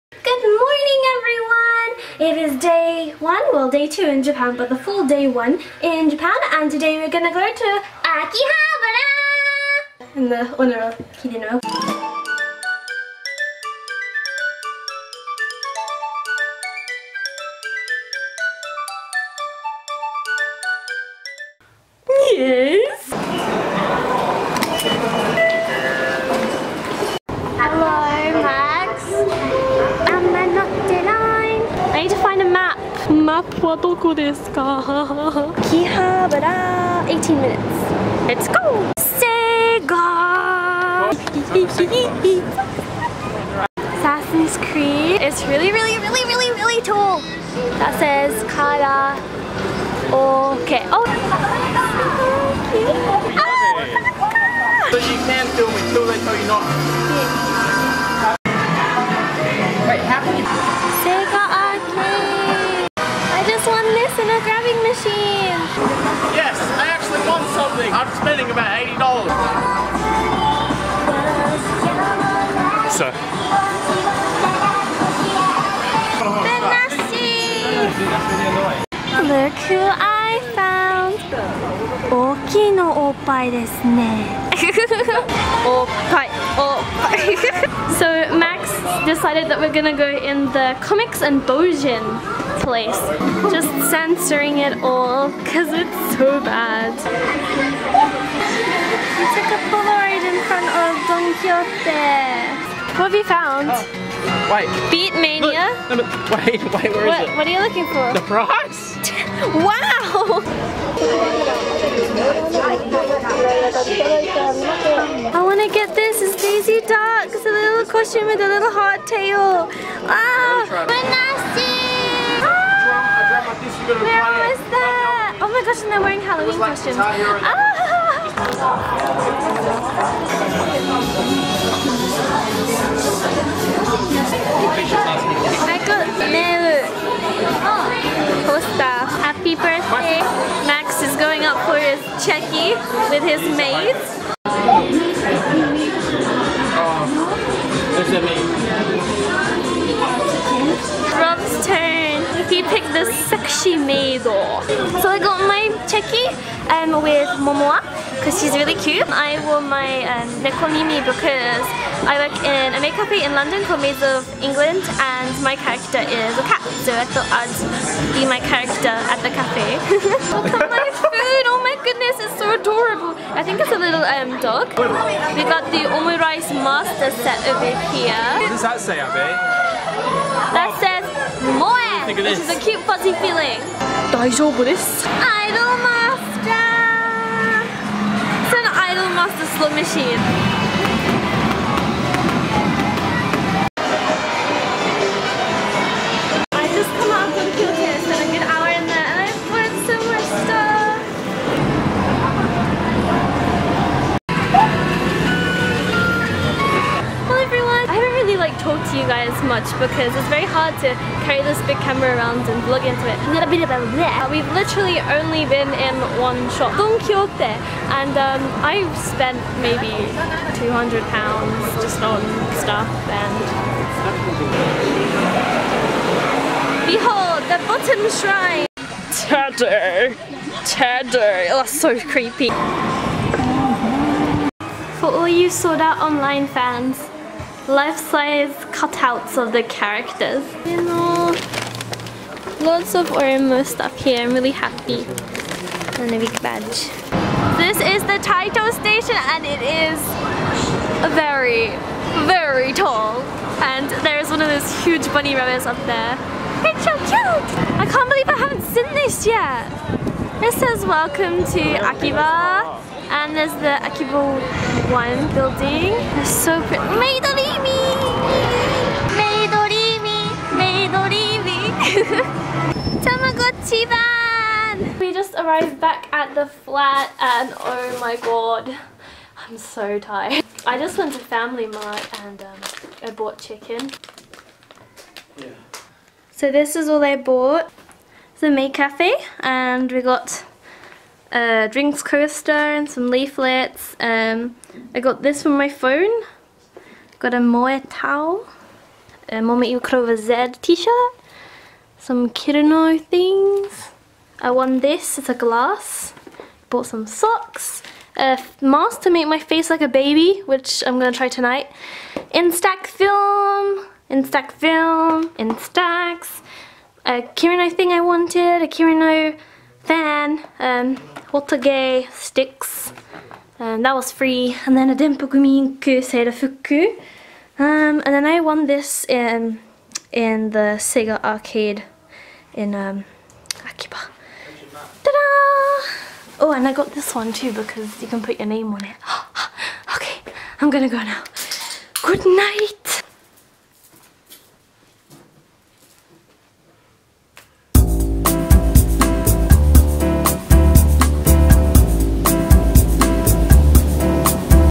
Abipop in Japan ♬*゜| Day 1 - Akihabara
Hello Everyone~~~♪

This is the 2nd video of 7 from my trip to Japan with my friends Max and Xantia~!!!

In this video I visit Japan's "Electric Town" Akihabara~! One of the places with the most Anime, Manga, Games and more~! We go to a famous maid café called "@Homecafe" too!

I hope you enjoy all the upcoming videos!
Thank you for your continuous support~!! I am over the moon~!

Don't forget to give the video a thumbs up and a share~!! ♡

Abi pop

Abipop

Music Sourced From:
☆ Epidemic Sounds
☆ Conte-de-fees